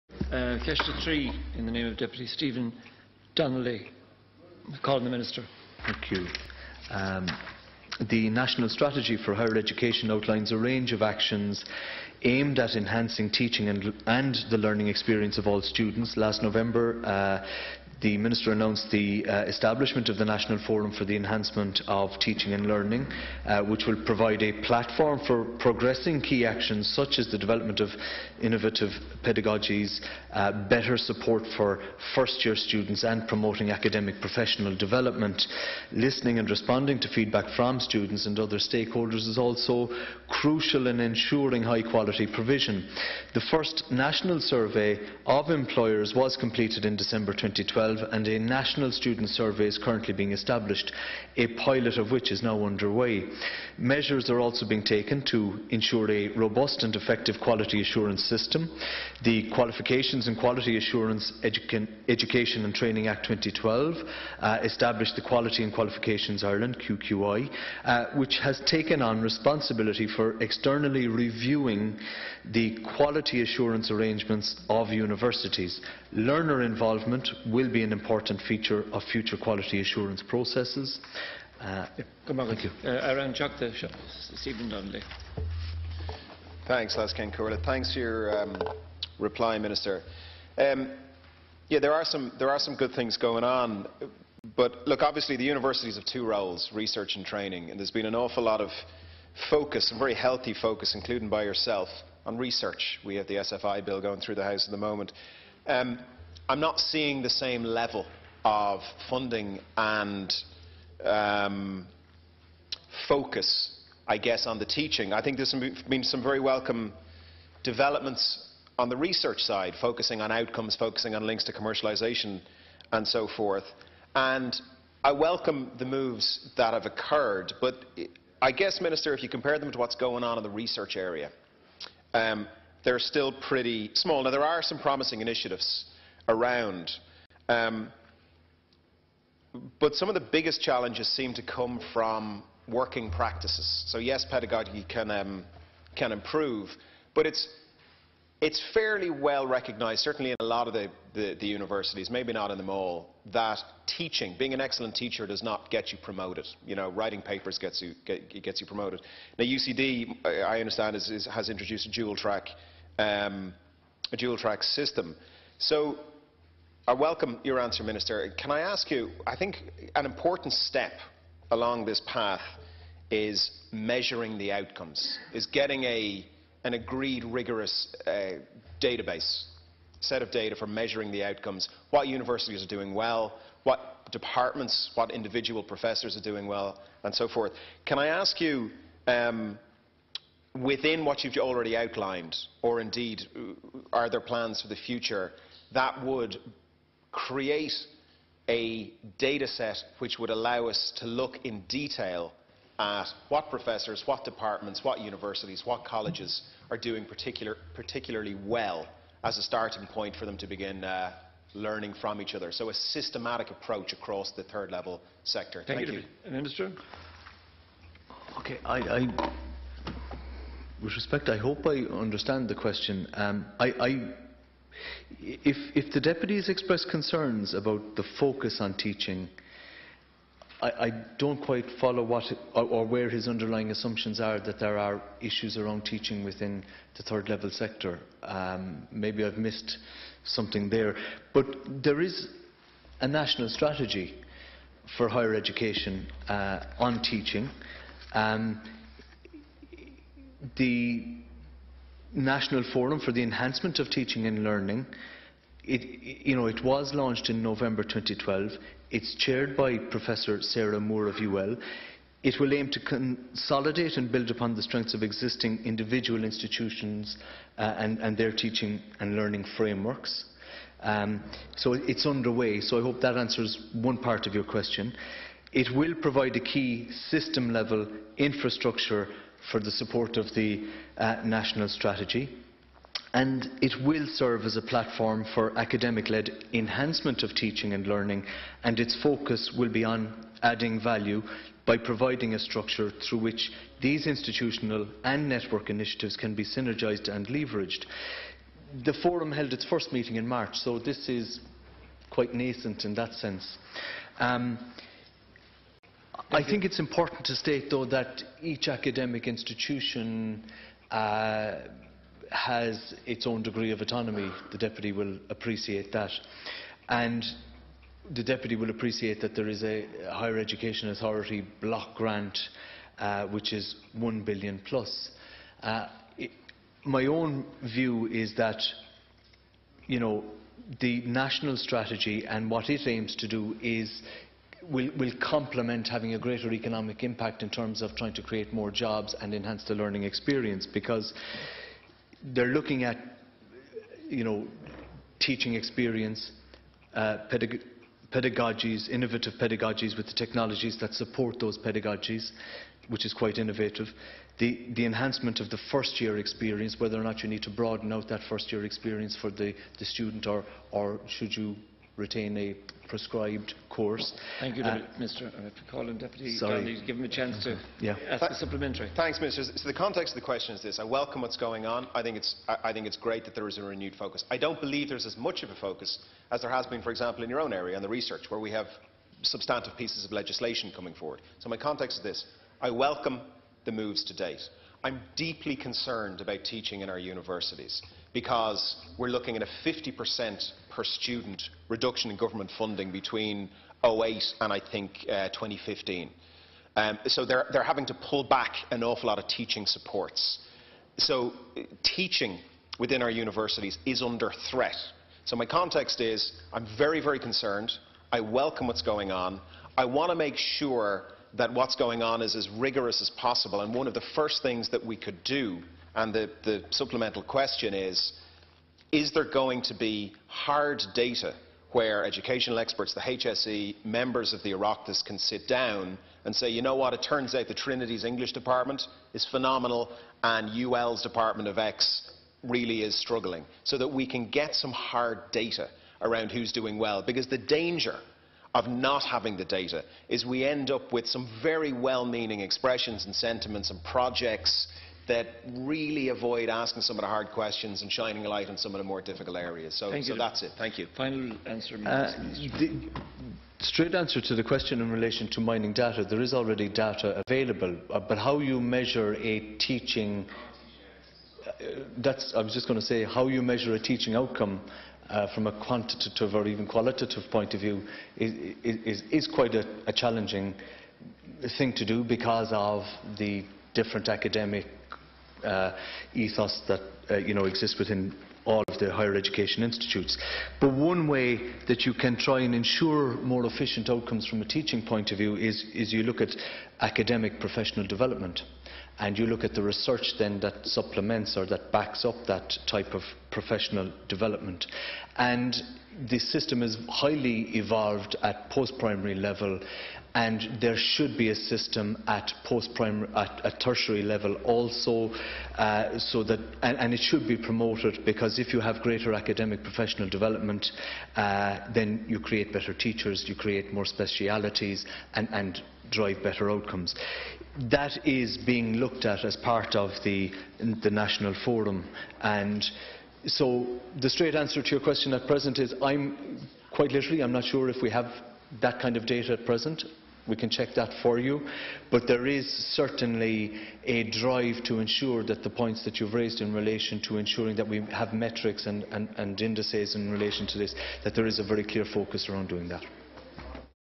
Talking teaching in our Universities
On April 17, 2011, I asked the Minister for Education what work has been done to assess, analyse and improve teaching outcomes in Ireland's universities and what tangible actions have been planned for the next 12 months as a result. Minister Quinn was in London for Margaret Thatcher's funeral, so Junior Minister Sean Sherlock gave a response.